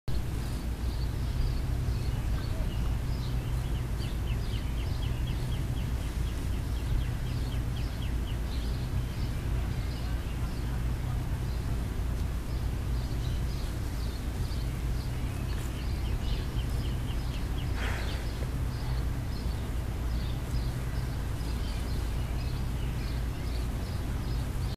Just a squirrel trying to get a nut
Seen this little guy while having a smoke